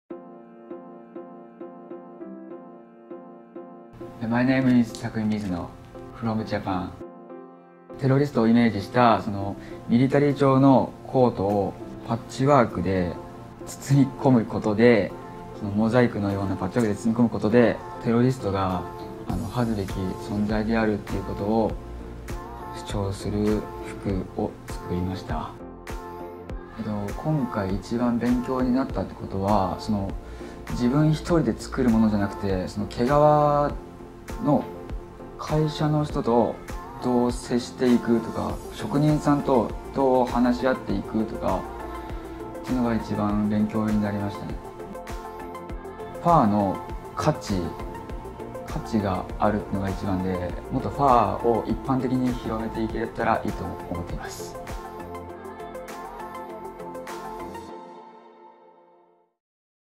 (Asia Remix 2018) Finalist Interview_ Takumi Mizuno (Japan)
Japanese finalist, Takumi Mizuno's interview.

Taking inspiration and making an effort in diminishing terrorism, Mizuno designed his work based on a patchy design with tones of brown and yellow.

Mizuno competed in the Menswear category, styling his work with a bold yellow beanie and street styled yellow utility belt.